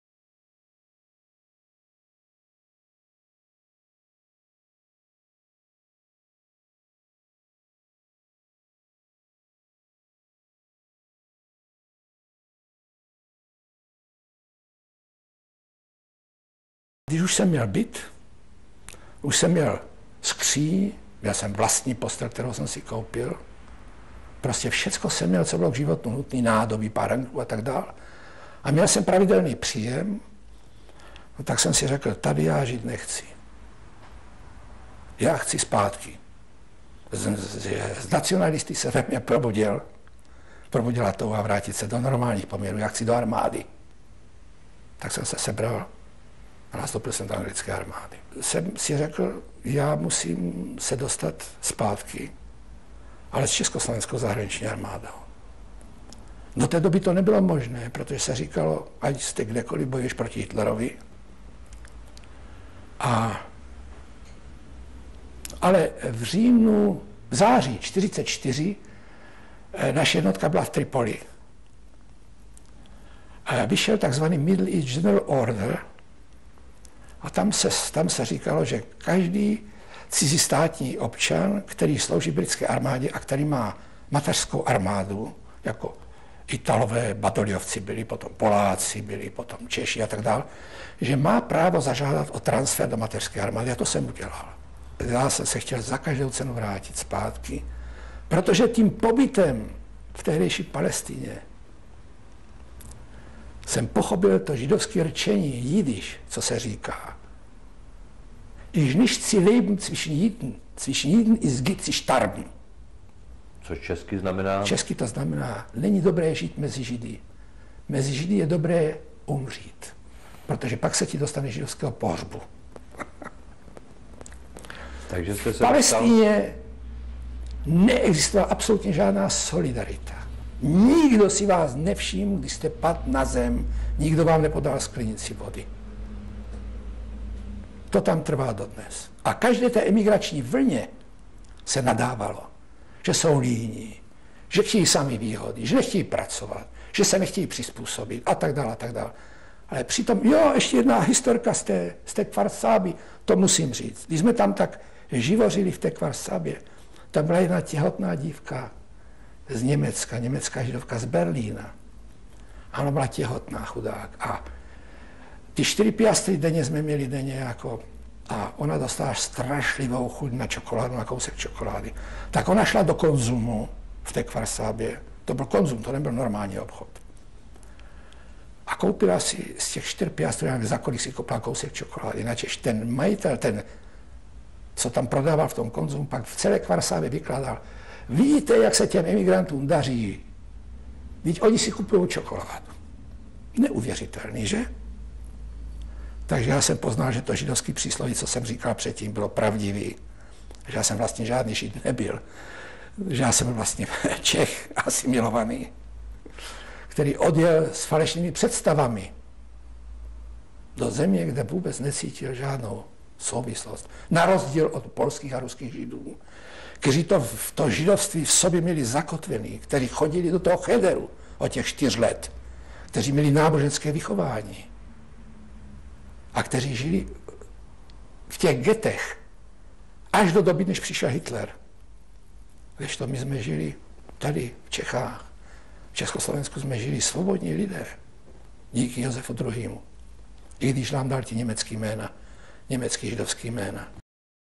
Gregor Kurt - Palestina a zahraniční armáda
Kurt Gregor (1918) během svého pobytu v Palestině se na podzim 1944 přihlásil do jednotky československé zahraniční armády, aby se mohl vrátit zpět do Československa.

klíčová slova:
Palestina, zahraniční jednotky.

Zařazení do tématického okruhu projektu Naši nebo cizí?
Doma na cestě? Migrace a uprchlíci